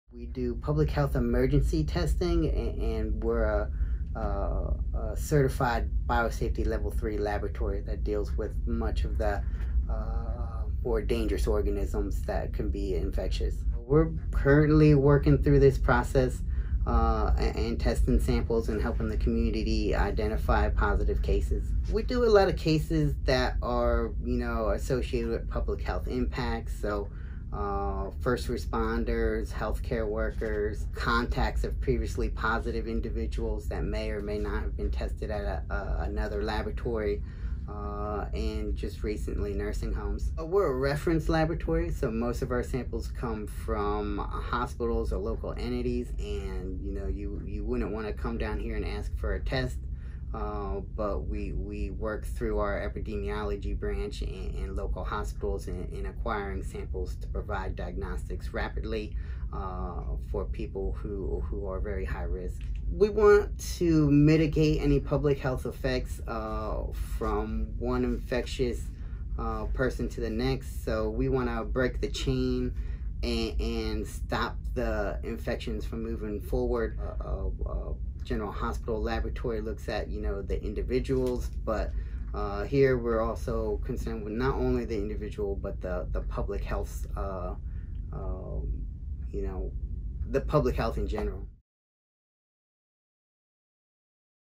Containing the Spread: How Dallas County COVID-19 Testing Lab Tests New Virus
Lab supervisor Daniel Serinaldi speaks from at the bioterrorism laboratory, which is part of the CDC’s Laboratory Response Network, at the Dallas County Health and Human Services building in Dallas on Monday, April 7, 2020. This lab has a 24-hour turnaround time for analyzing COVID-19 test kits for high-contact individuals, such as nursing homes residents, medical staff and first responders. “We test less kits, but we have a higher rate of positives,” Serinaldi said. “But, we would hope it has more impact because of the types of cases we’re testing.” (Lynda M. Gonzalez/The Dallas Morning News)